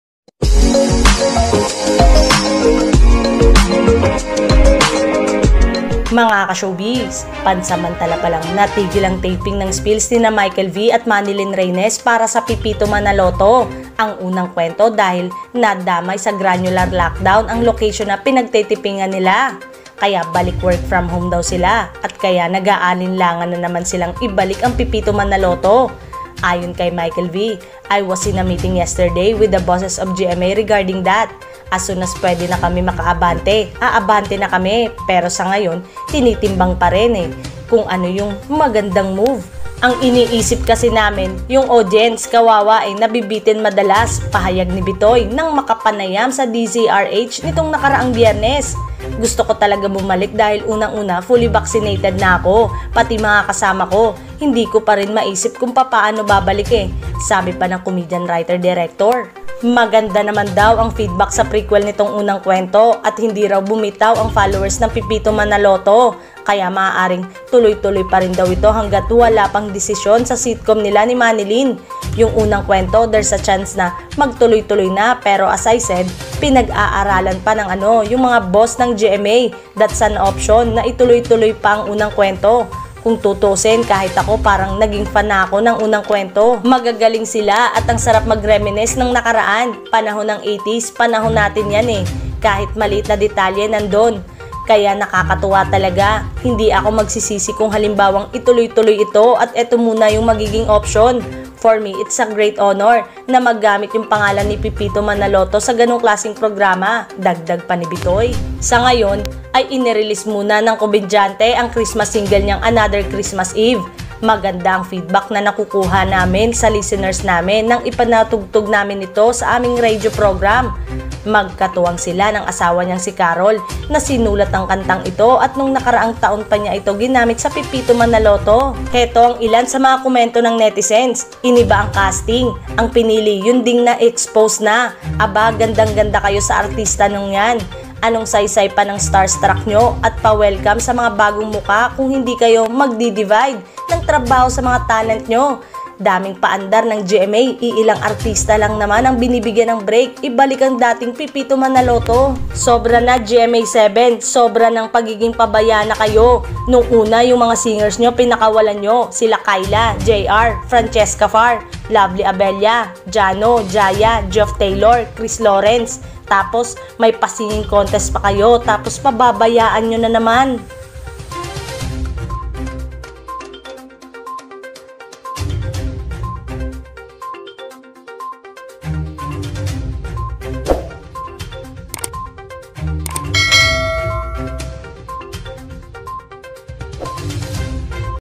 MICHAEL V UMAASA PA RIN SA DESISYON NG GMA SA MULING PAG ERE NG KANILANG SHOW
Pansamantala palang natigil ang taping ng spiels nina Michael V at Manilyn Reynes para sa Pepito Manaloto: Ang Unang Kuwento dahil nadamay sa granular lockdown ang location na pinagteypingan nila.

Kaya balik "work from home" daw sila, at kaya nag-aalinlangan na naman silang ibalik na ang Pepito Manaloto.